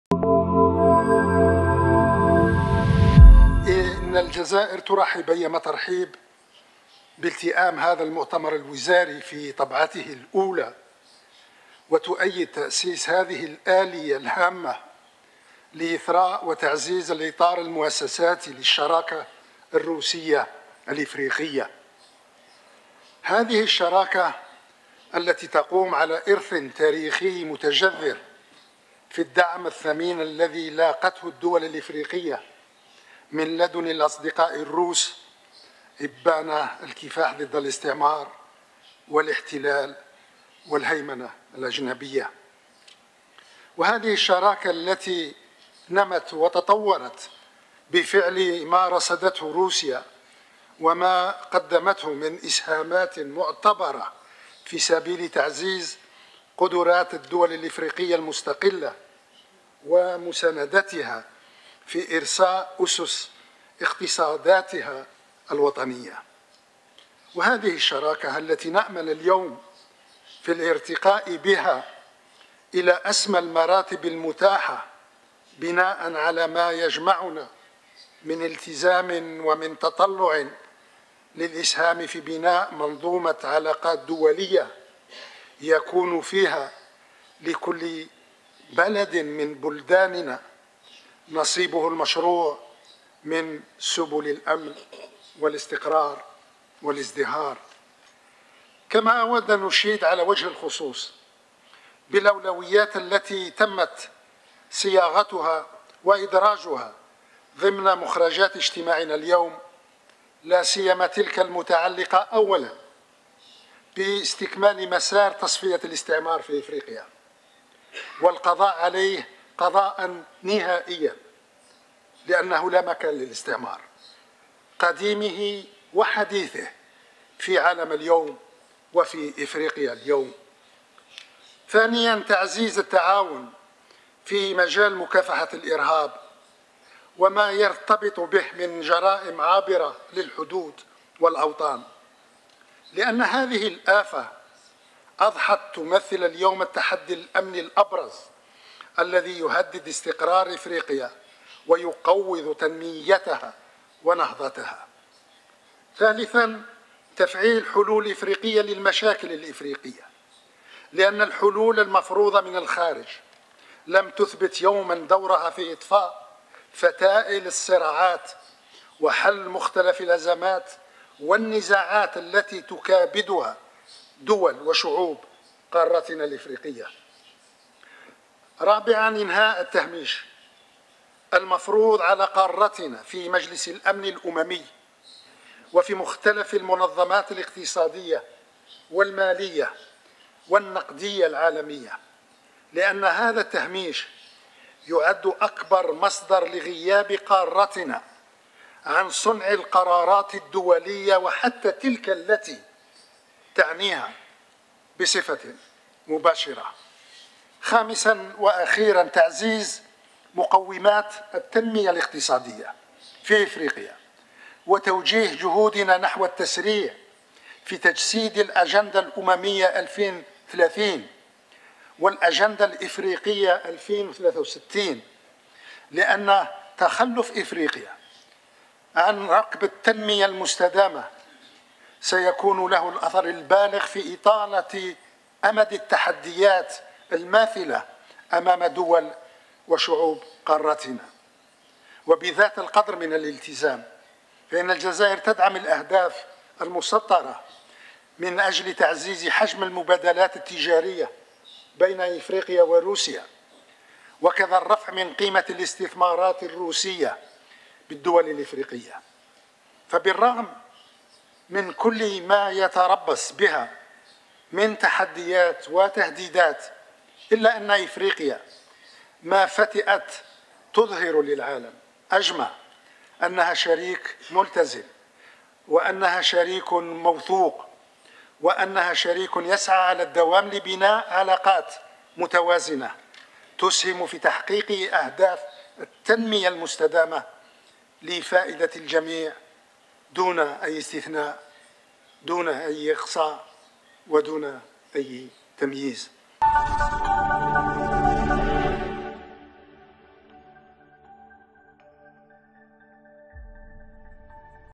سوتشي : السيد عطاف يشيد بالأولويات المدرجة ضمن مخرجات المؤتمر الوزراري للشراكة الإفريقية - الروسية
سوتشي(وأج) - أشاد وزير الشؤون الخارجية والجالية الوطنية بالخارج أحمد عطاف اليوم الأحد بمدينة سوتشي الروسية بالأولويات المدرجة ضمن مخرجات المؤتمر الوزاري للشراكة الإفريقية-الروسية لاسيما تلك المتعلقة باستكمال مسار تصفية الاستعمار في إفريقيا وإنهاء التهميش المفروض على القارة في مجلس الأمن الأممي.